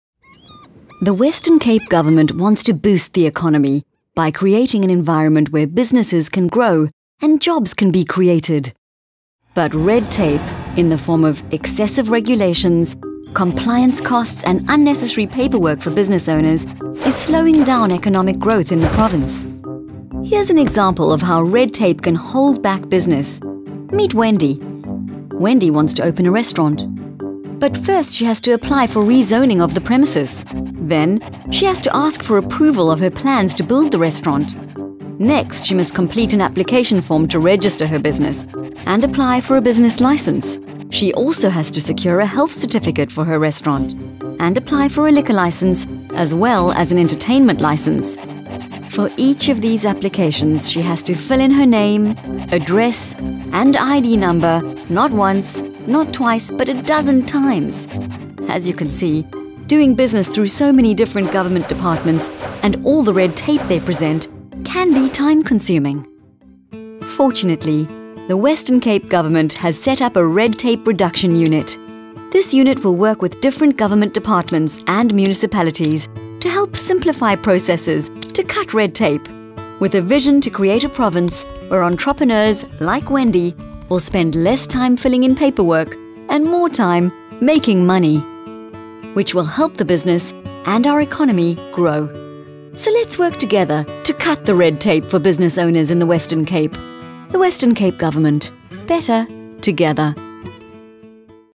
Cutting Red Tape Starts with You
The Western Cape Government's Red Tape Reduction unit, helps simplify doing business in the province by offering support to entrepreneurs and business owners.